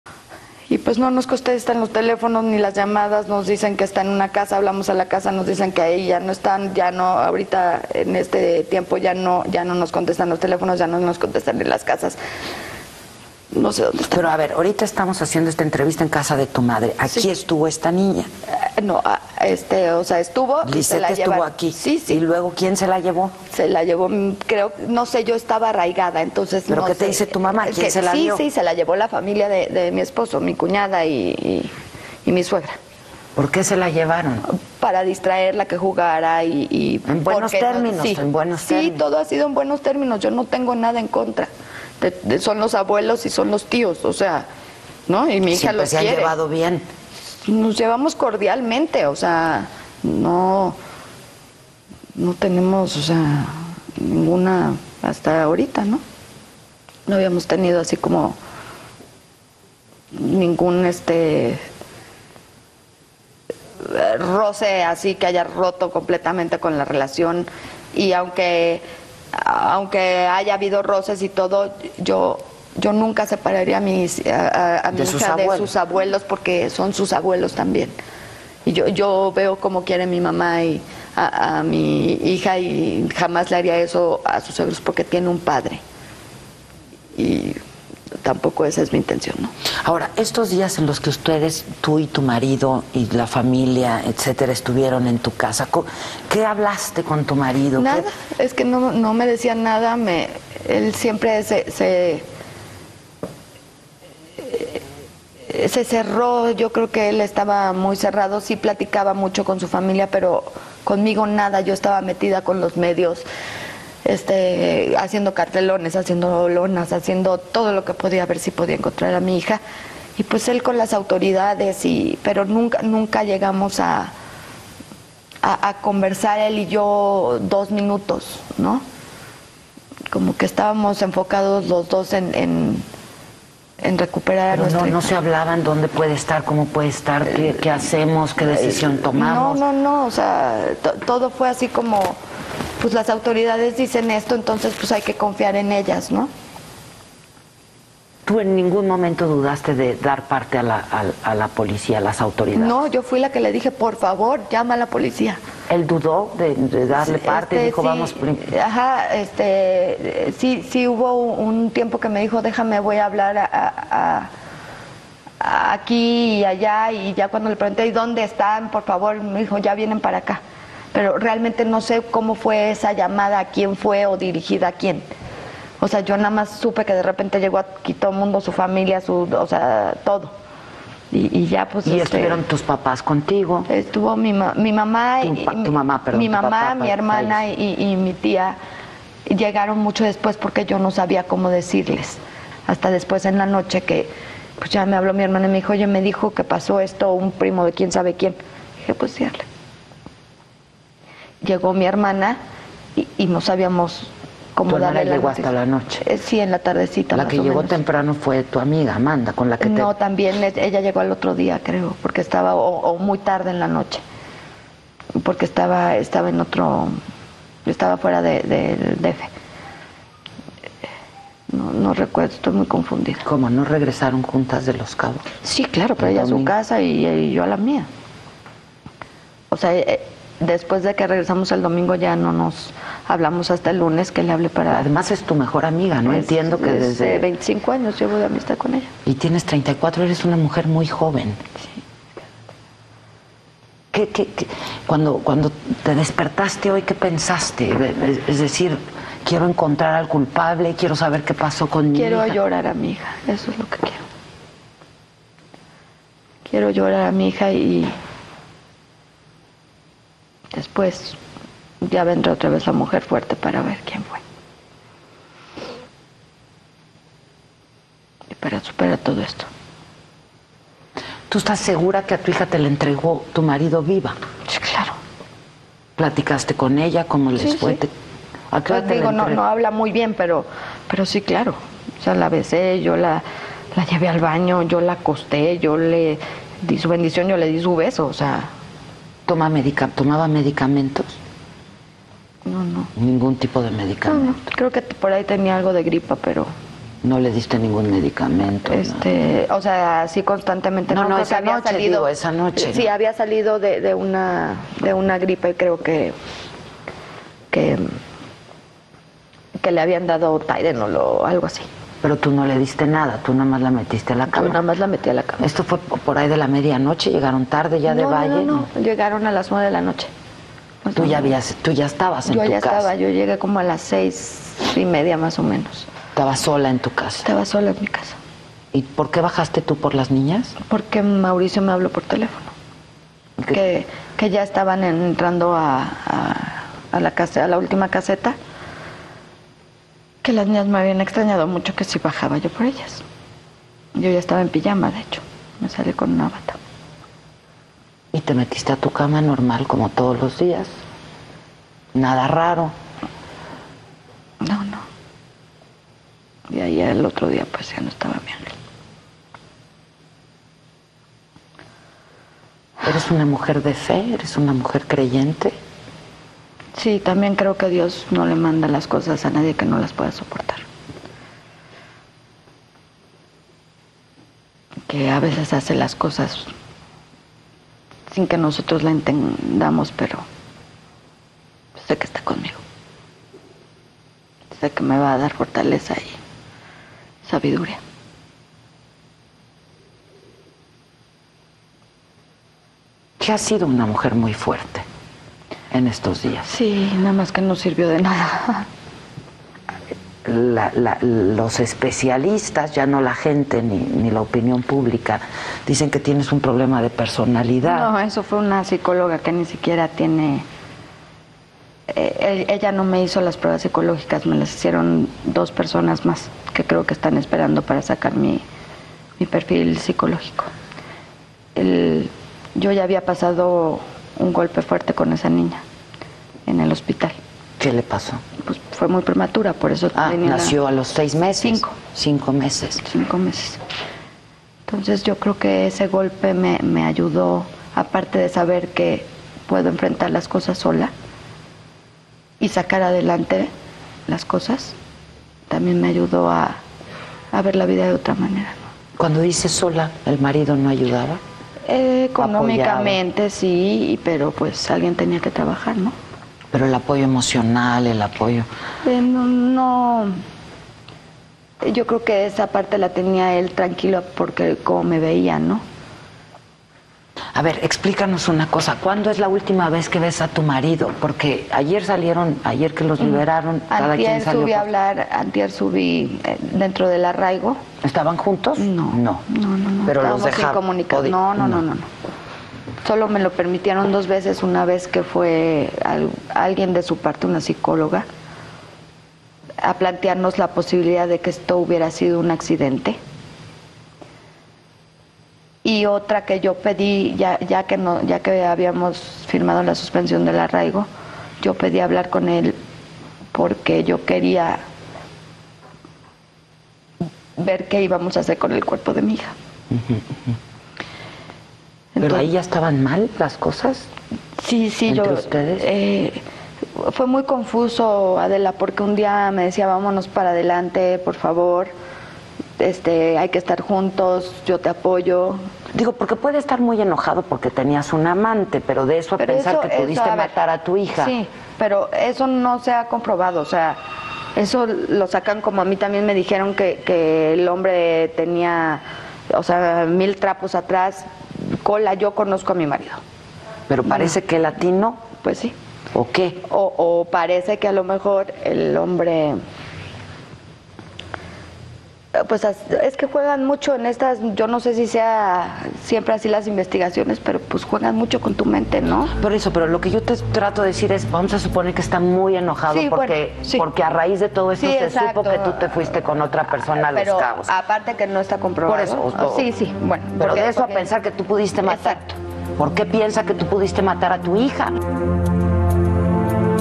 2da parte de la entrevista Adela Micha a Lisette Farah 2/4 martes 06 abril 2010
La segunda parte de la entrevista de Adela Micha a Lisette Farah trasmitida el martes 06 abril 2010